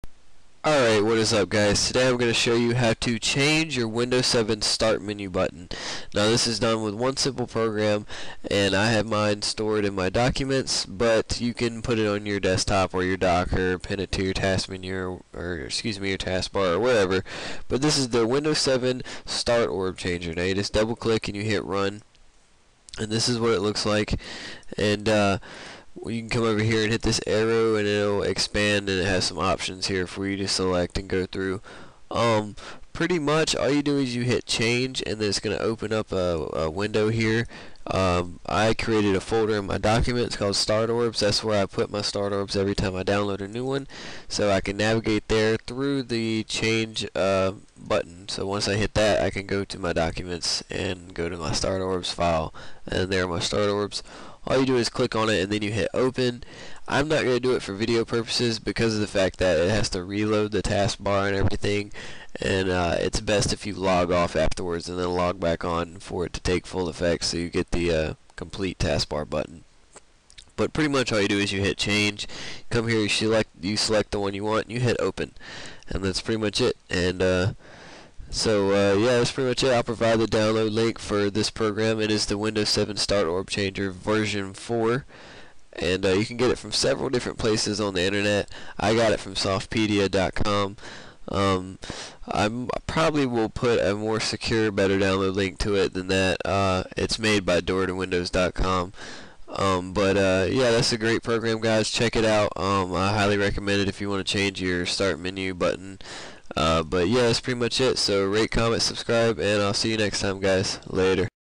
Windows 7 Start Orb Changer TUTORIAL!
Alright guys this is a great windows 7 start orb changer and its free!. links will be below :)